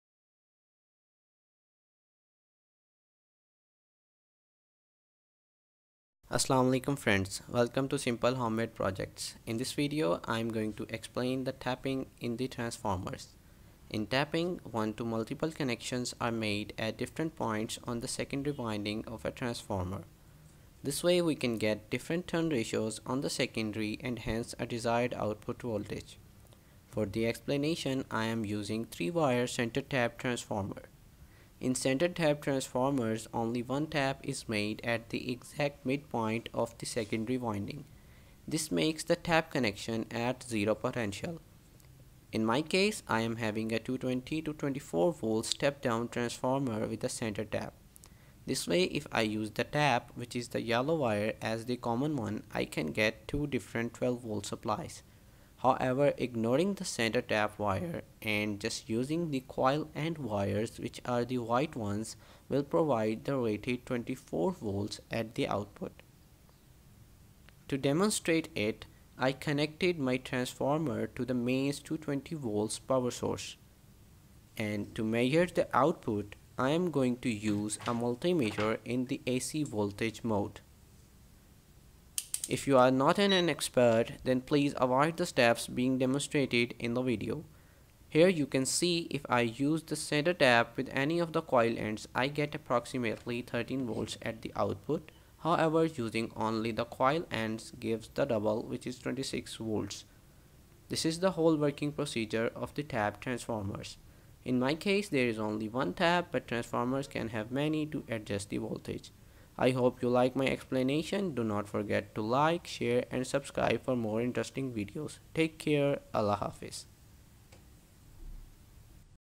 How to Use a 3-Wire Transformer || Center Tapped Transformer || How Tapping works in Transformers?
Assalam O Alikum Friends, in this video I have explained the working and usage of transformer taps along with the demonstration.
I have used a 220 to 24-volt step-down transformer which has only one center tap. Transformers having only one tap exactly in the middle of the secondary winding are known as Center Tapped Transformers. We can use it to either take a single or double 12 Volts output. Alternatively, without using the Tap, we can get 24 Volts.